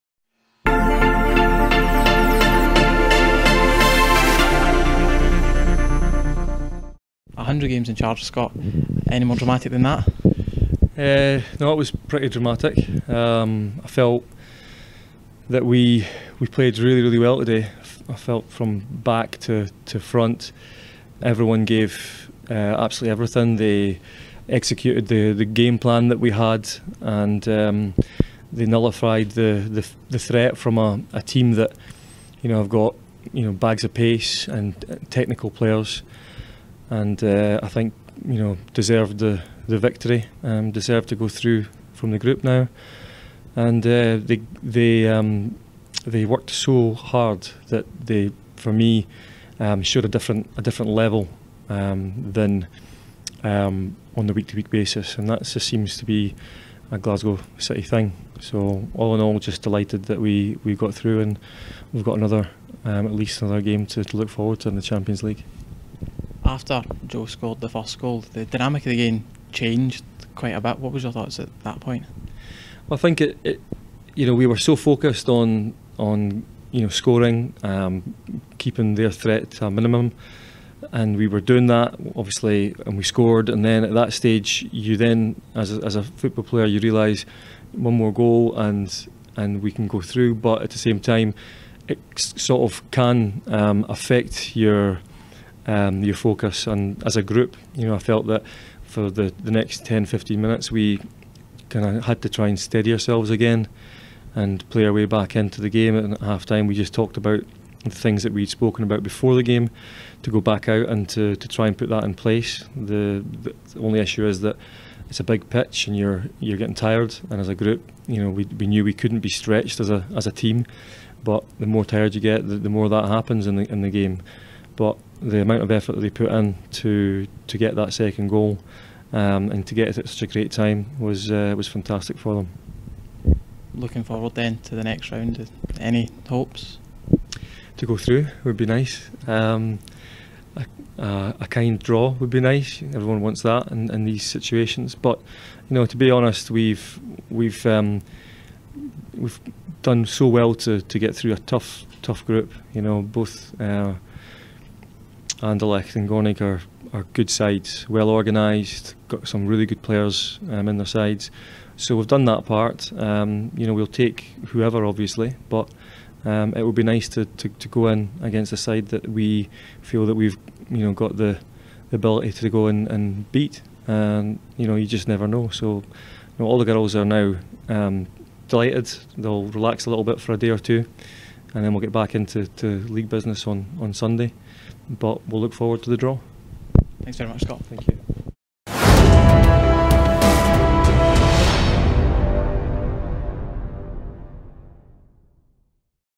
REACTION | Scott Booth (GOR 0-2 GLA) - 13/8/18
Reaction from Scott Booth as Glasgow City make the last 32 of the UWCL.